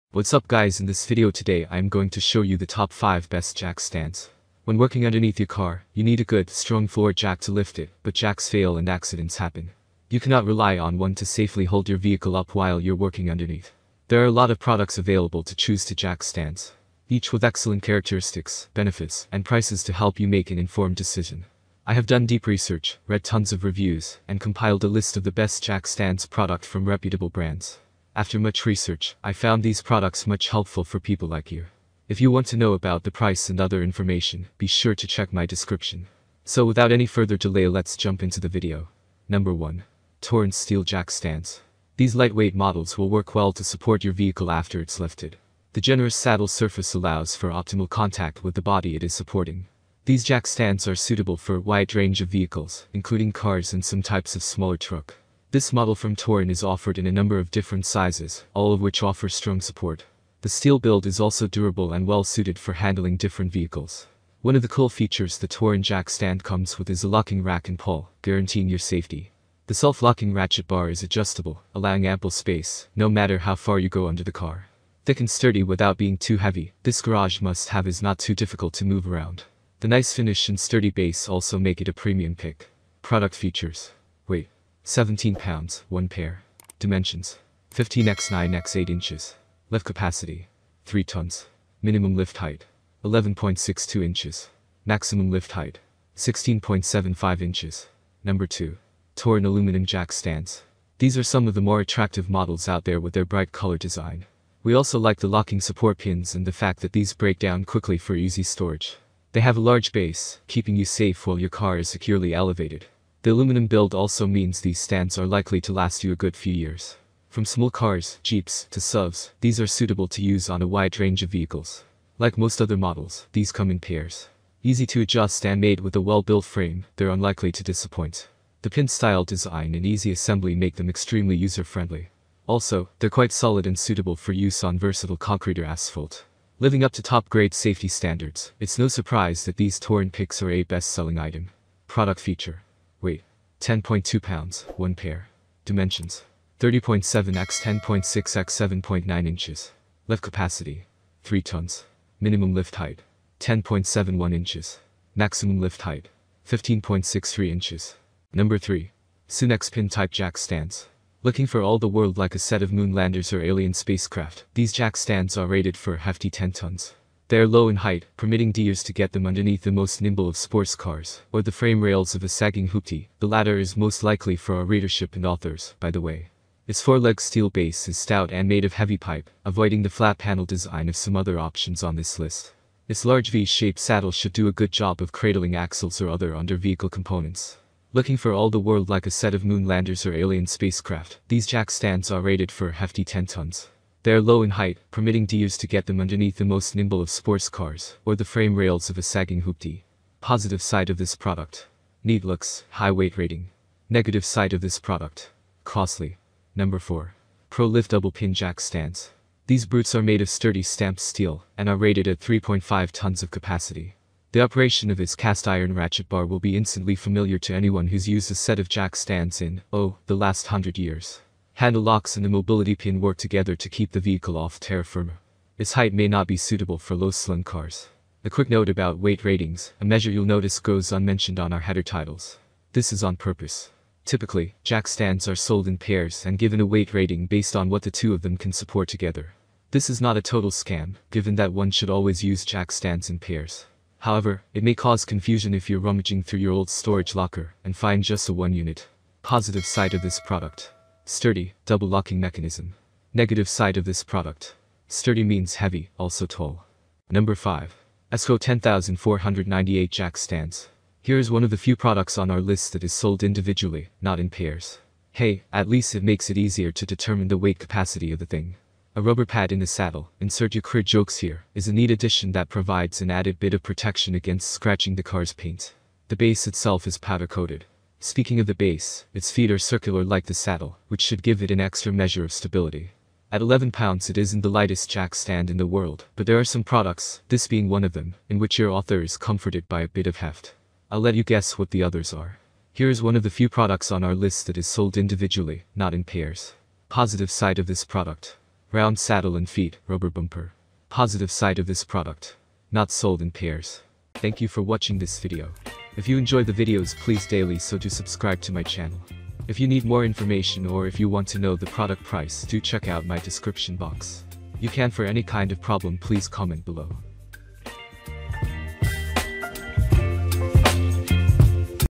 Top 5 Best Jack Stands 2023 🔥
In This Video, Best Jack Stands 2023 🔥
📌 Product Link 📌
✅ 1.  Product Name: Torin Steel Jack Stands
📌 Product Link :
✅ 2.  Product Name: Torin Aluminum Jack Stands
📌 Product Link :
✅ 3. Product Name: Sunex  Pin Type Jack Stands
📌 Product Link :
 ✅ 4.  Product Name: Pro-Lift Double Pin Jack Stands
📌 Product Link :
✅ 5. Product Name: Esco 10498 Jack Stands
📌 Product Link :

("Paid Link")